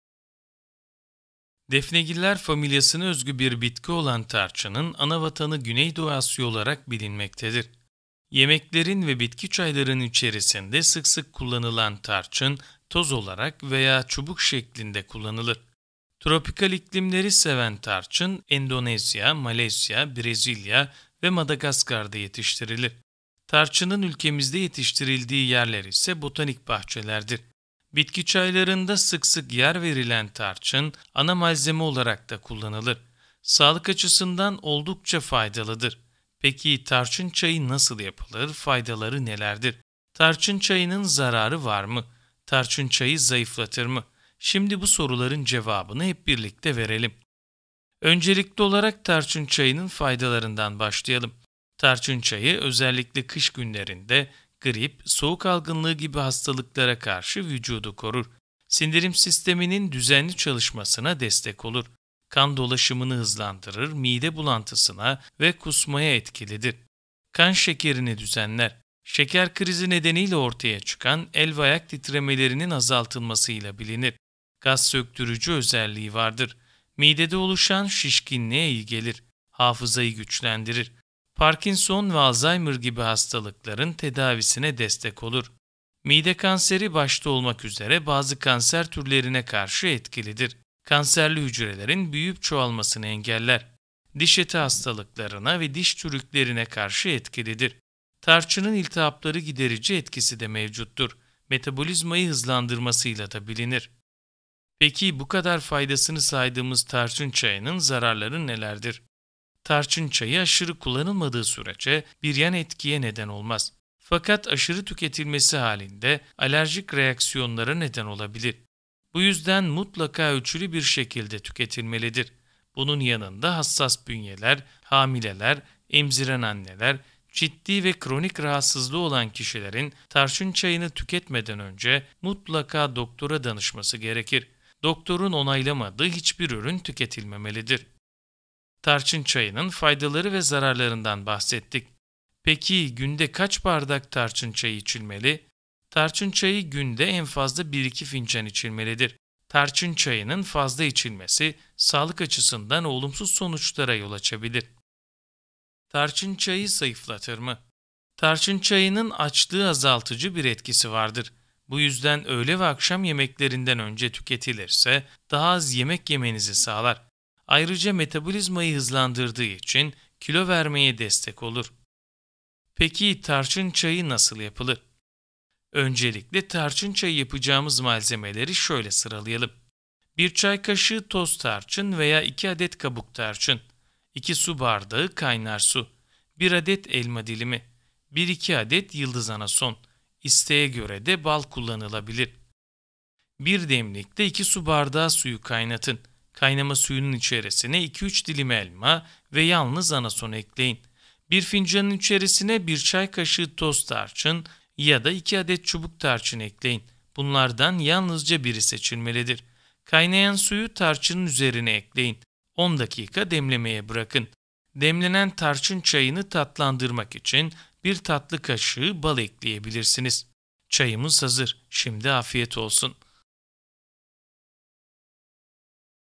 Tarçın çayı nasıl yapılır? Tarçın çayının faydaları nedir? Tarçın çayı zayıflatır mı?
Tarçın çayının faydaları nelerdir? Evde tarçın çayı nasıl yapılır? Tarçın çayı ne kadar içilmeli? Tarçın çayının zararı var mı? Tarçın çayı zayıflatır mı?

Defnegiller familyasına özgü bir bitki olan tarçının ana vatanı Güneydoğu Asya olarak bilinmektedir. Yemeklerin ve bitki çaylarının içerisinde sık sık kullanılan tarçın, toz olarak veya çubuk şeklinde kullanılır.  Peki, tarçın çayı nasıl yapılır, faydaları nelerdir? Tarçın çayının zararı var mı? Tarçın çayı zayıflatır mı? Şimdi bu soruların cevabını vereceğiz.

00:00 Tarçın nerede yetişir?
00:47 Tarçın çayının faydaları nedir?
01:44 Tarçın çayının zararları var mı?
02:18 Tarçın çayı ne kadar içilmeli?
02:35 Tarçın çayı zayıflatır mı?
02:52 Tarçın çayı nasıl yapılır?